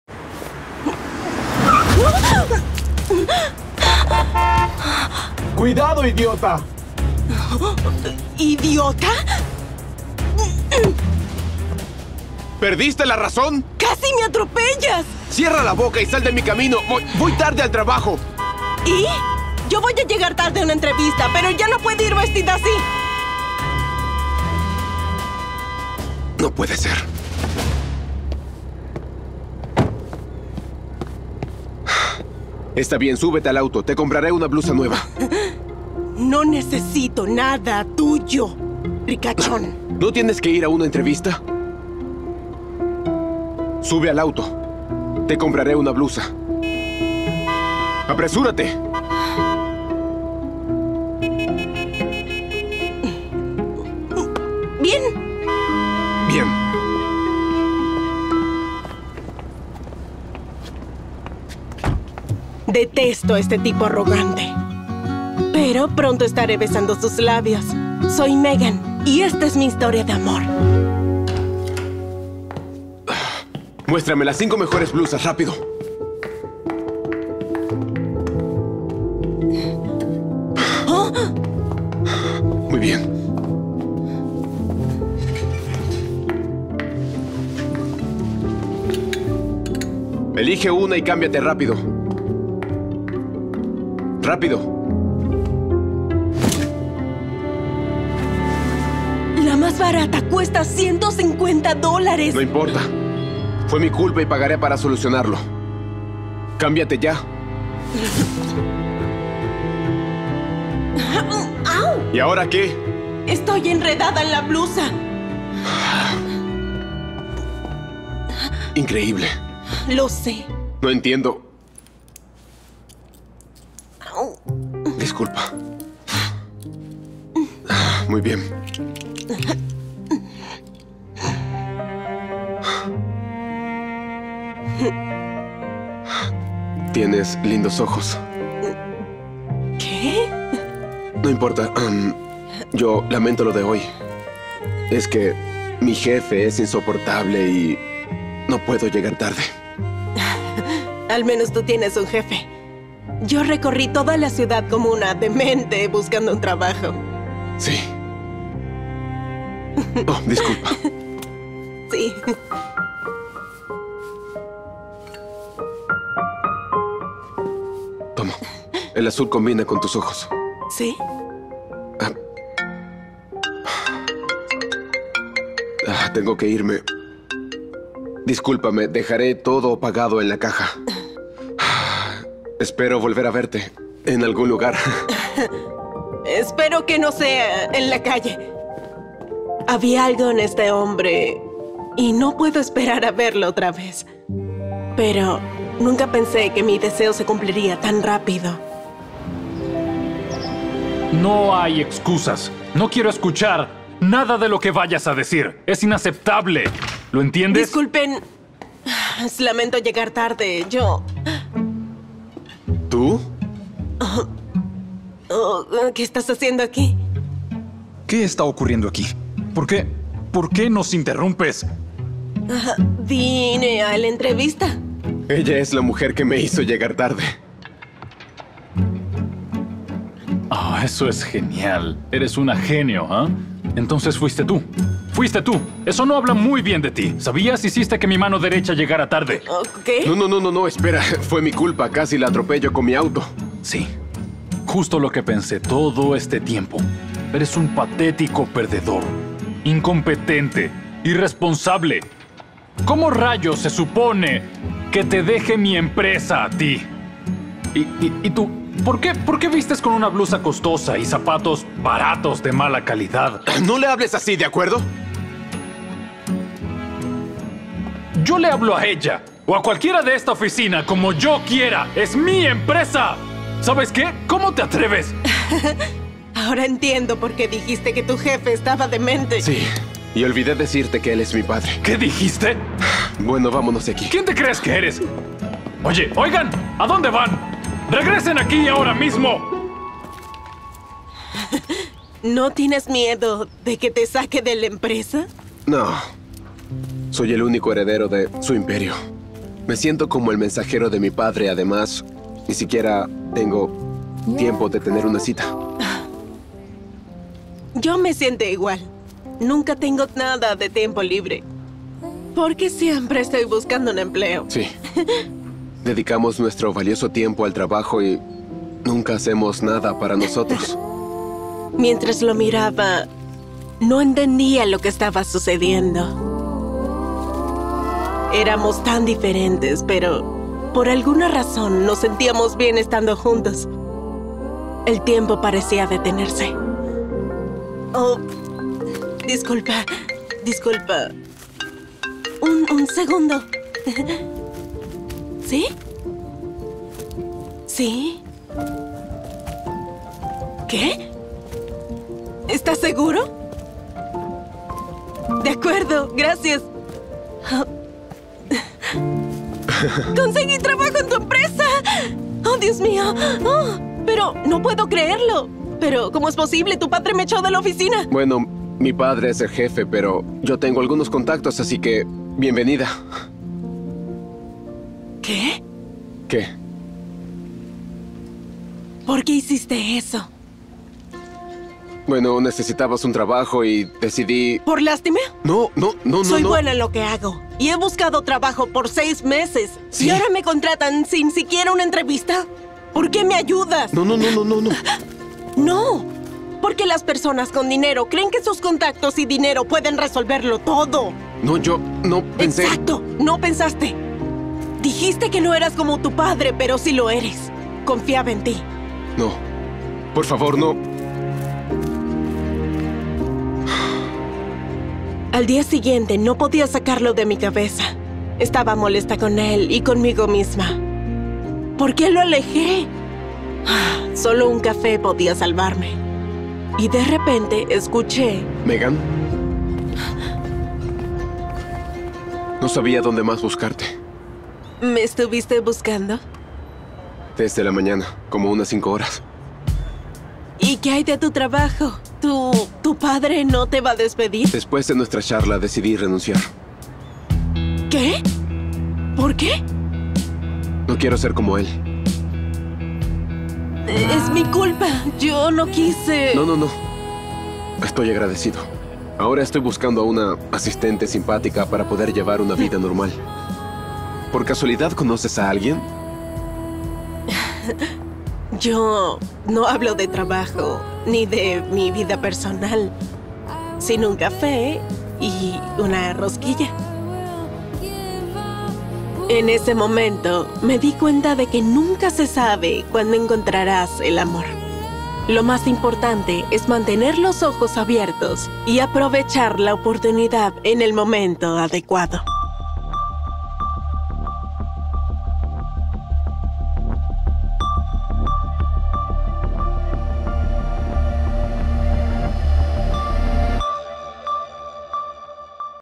UN MILLONARIO me GOLPEÓ
Estaba de camino a una entrevista cuando, de repente, un auto me golpeó. El conductor fue un patán, pero, al final, se ofreció a ayudarme. Ahora, creo que estoy enamorada de él ...